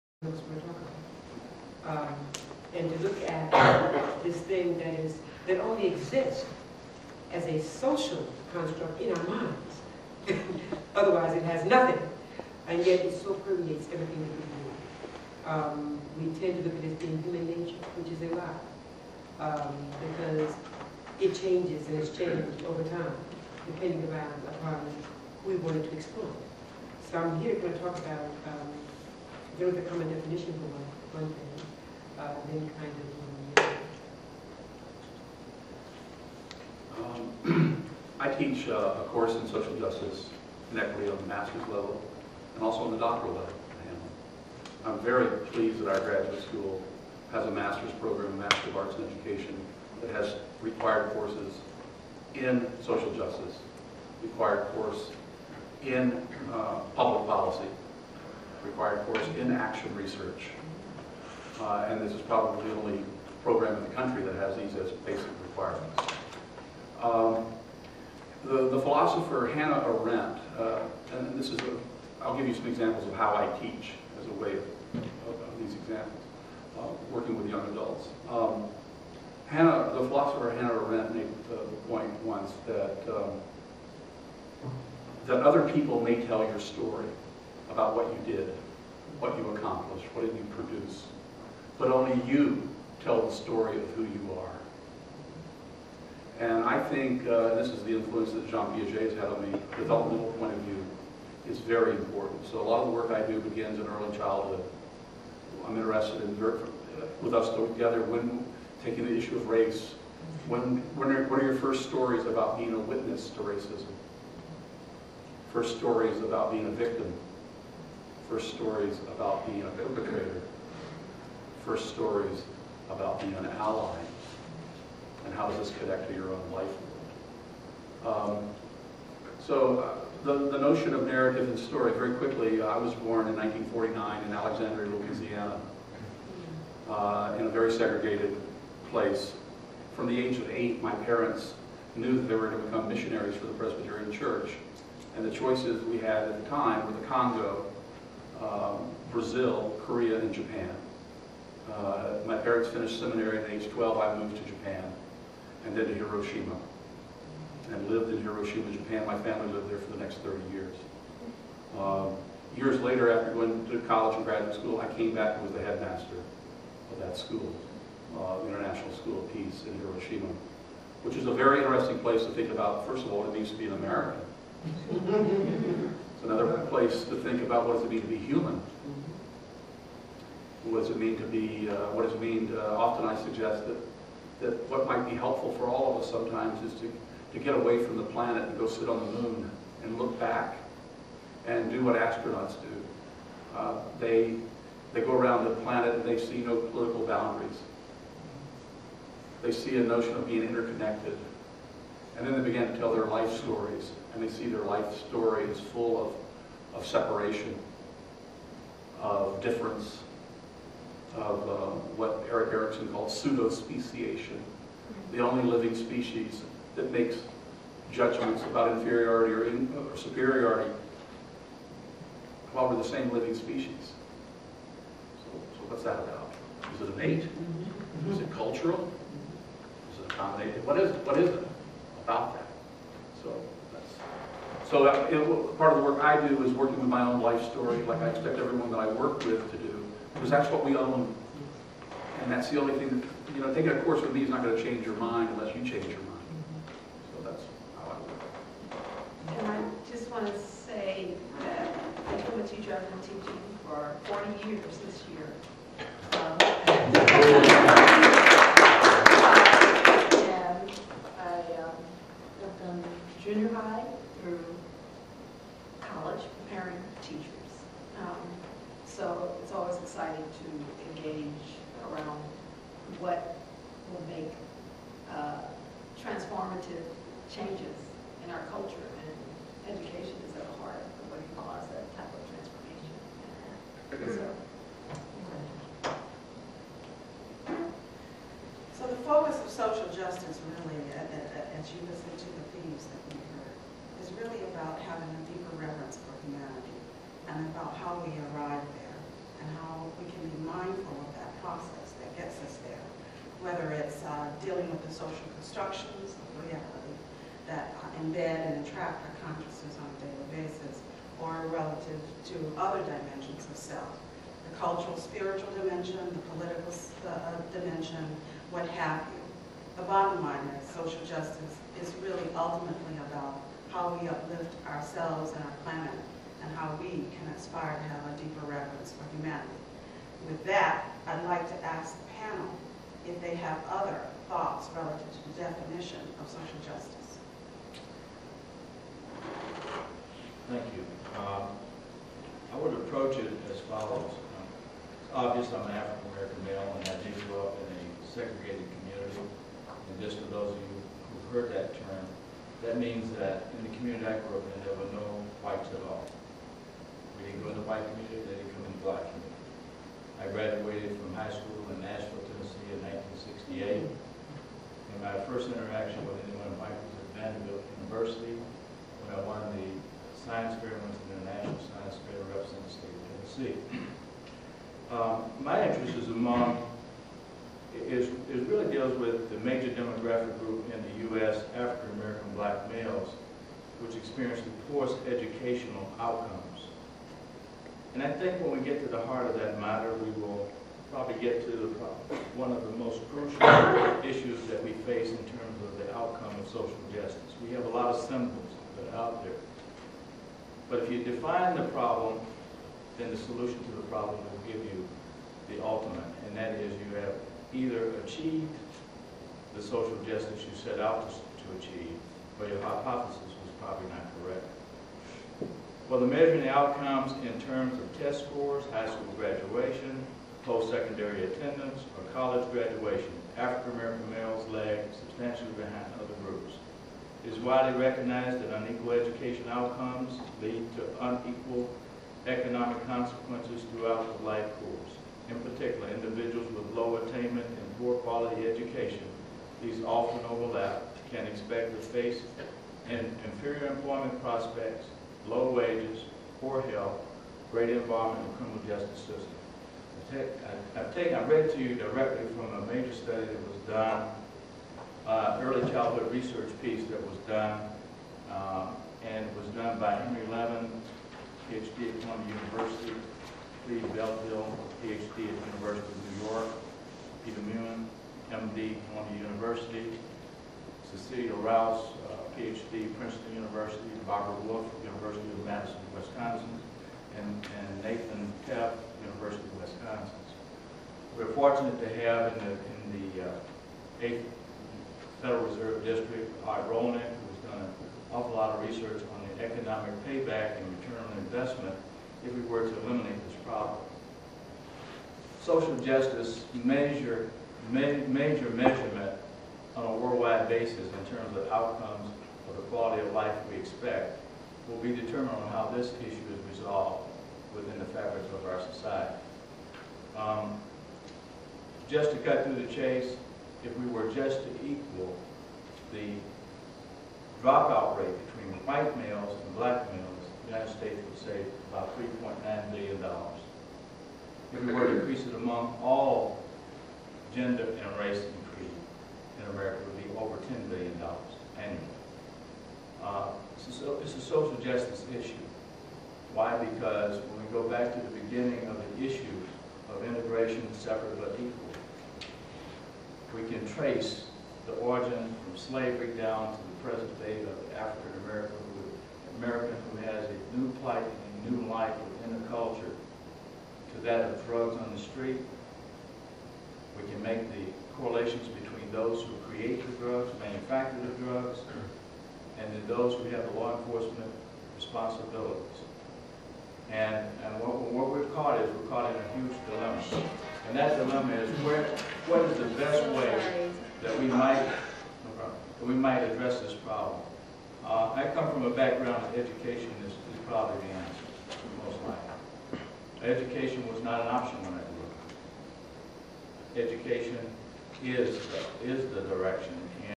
UACT Social Justice Panel - Nov. '09 (Part 2)
A panel discussion moderated by Dr. Sheila Wright, Dean of the Hamline School of
Education; with special guest panelist Georgette Norman, Director of the Troy
University Rosa Parks Museum, Montgomery, Alabama; and featuring Jesse Overton,
Chair of the Advisory Board, Hamline Universitys Center for Excellence in Urban
Teaching; Walter Enloe, Professor in Human Studies at Hamline University; and
Beverly Smith‐Davis, member of the Advisory Board of Hamline Universitys Center
for Excellence in Urban Teaching.